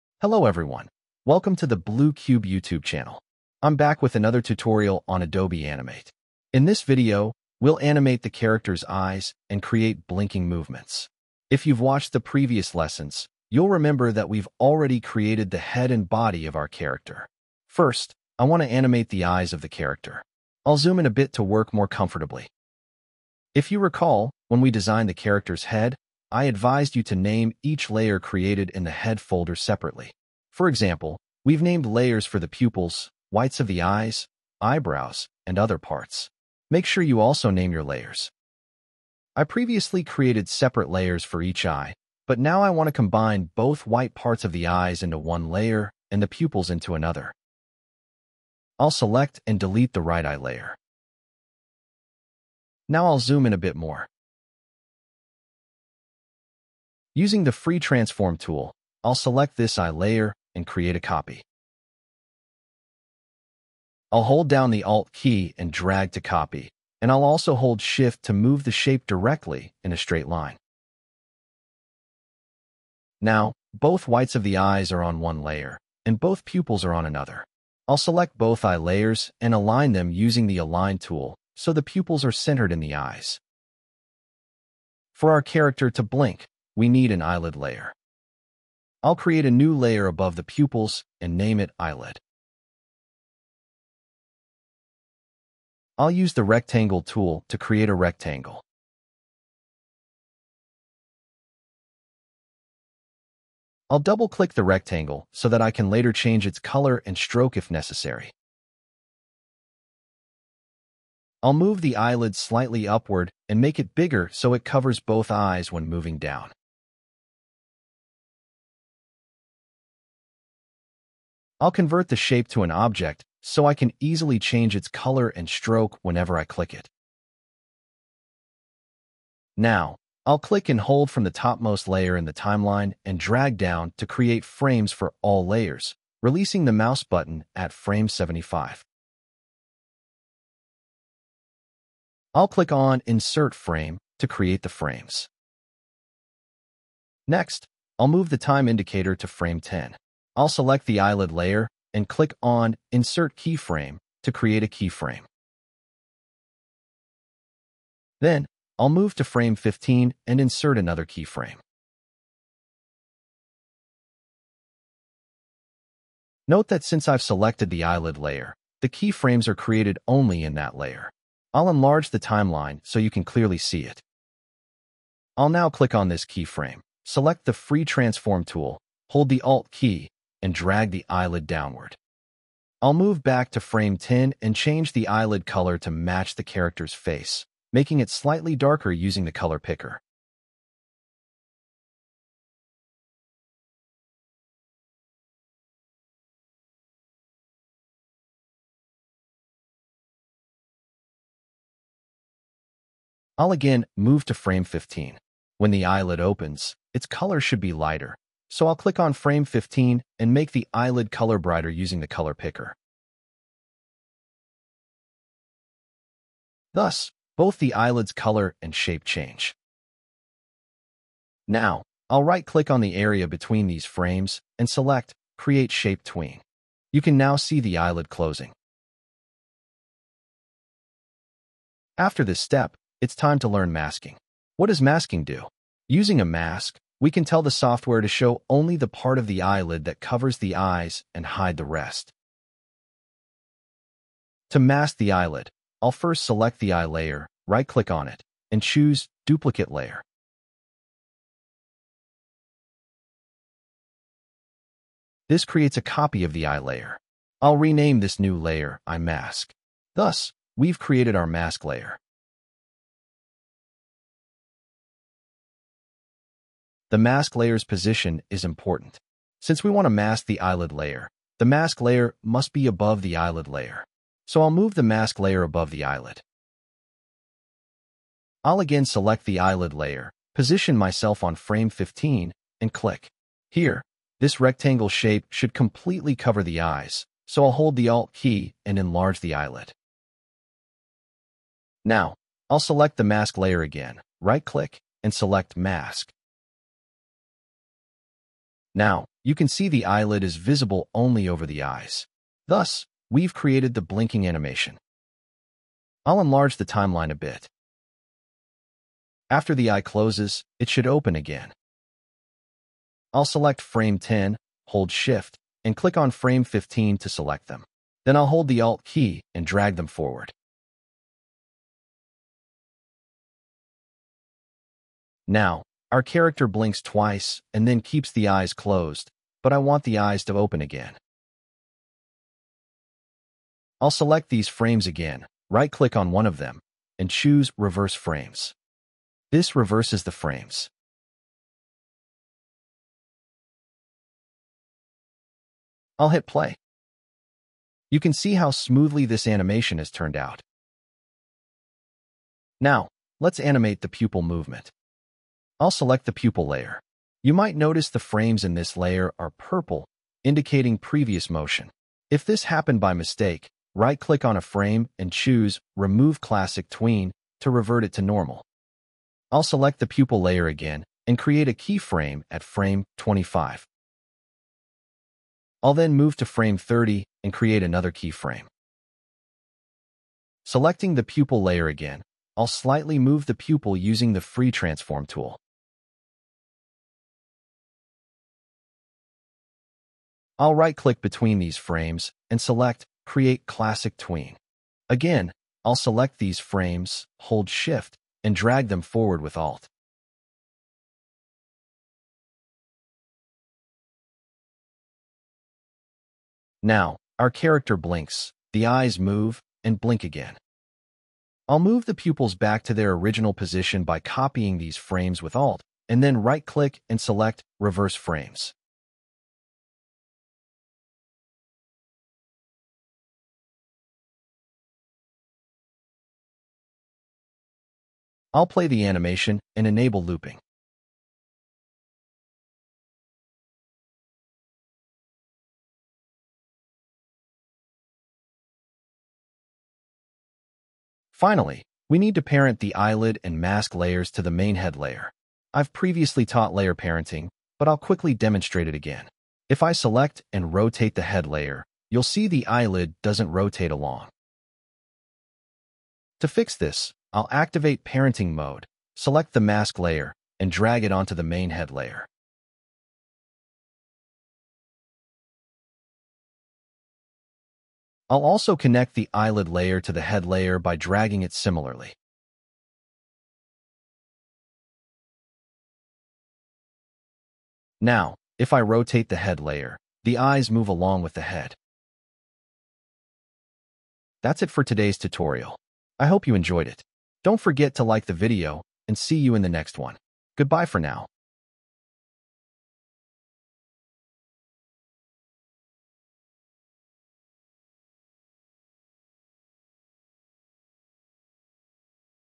Animate Blinking Eyes & Pupil Movement in Adobe Animate | Quick & Easy Tutorial
Want to bring your cartoon characters to life? In this tutorial, you'll learn how to animate realistic blinking and smooth pupil movement using Adobe Animate. With clear voice guidance and step-by-step instructions, this video is perfect for beginners and aspiring animators!

This tutorial covers the following topics

00:00 start
00:17 Animate Blinking Eyes
03:51  Masking in Adobe Animate
05:51  Pupil Movement